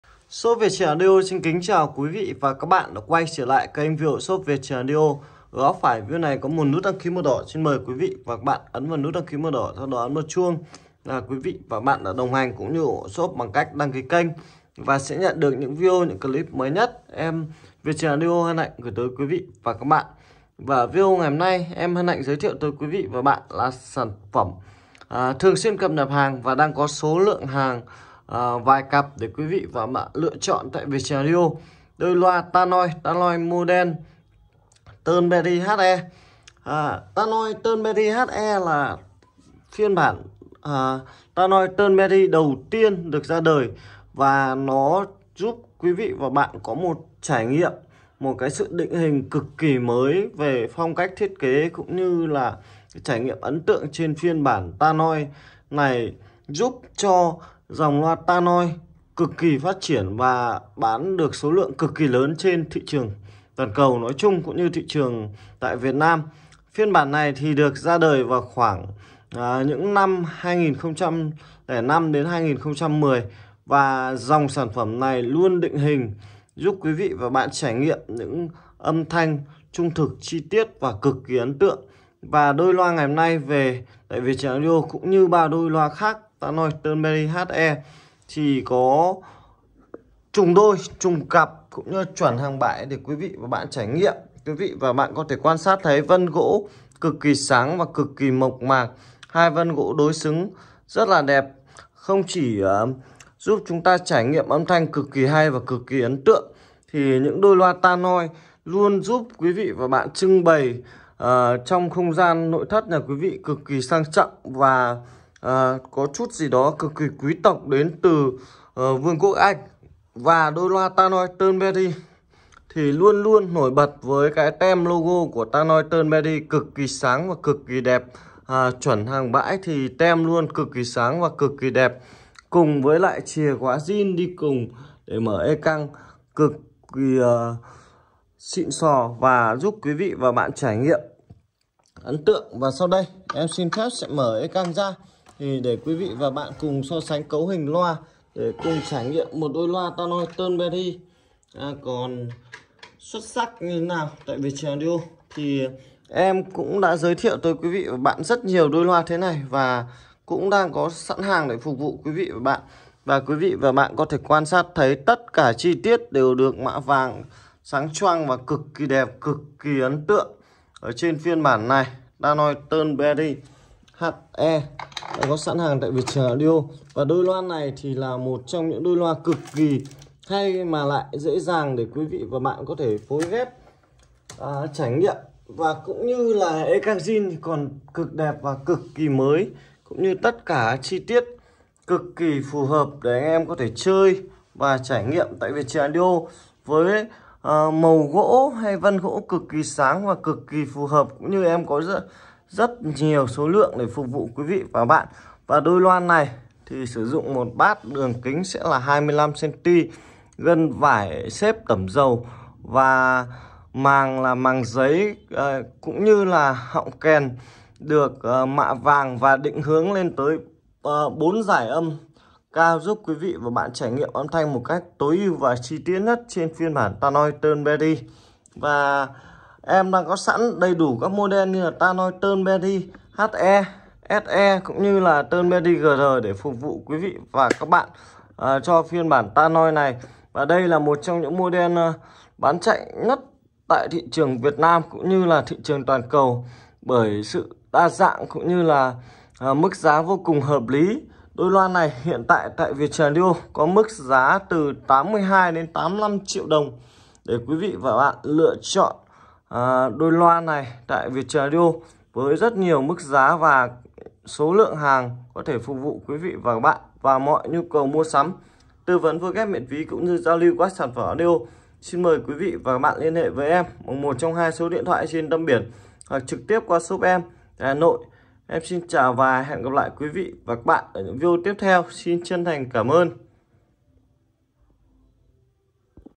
🔈🔈Shop về số lượng Loa Tannoy Turnberry HE Đẹp xuất sắc chỉ từ 80 triệu đồng aj❗️
Loa Tannoy Turnberry HE Đẹp xuất sắc
Hàng siêu thị Nhật về rất đẹp, nguyên zin tất cả. Loa Turnberry HE có kiểu dáng vintage, loa đồng trục kép bass 25Cm màng giấy, gân vải, phía trước có tinh chỉnh dải trung và cao, 2 khe thoát hơi hai bên mặt trước thùng loa. Loa có độ nhậy cao 93Db và trở kháng 8Ohm nên dễ phối ghép đa dạng amply. Tannoy Turnbery là dòng loa dễ chơi và dễ thanh khoản khi nâng đời và trao đổi, phù hợp cả nghe lẫn bày.
Giá: 82-85t
Shop Việt Trường Audio
Chất lượng tốt - Giá còn tốt hơn
Địa chỉ: Số 105, Ngõ 217 Trần Phú, Văn Quán, Hà Đông, Hà Nội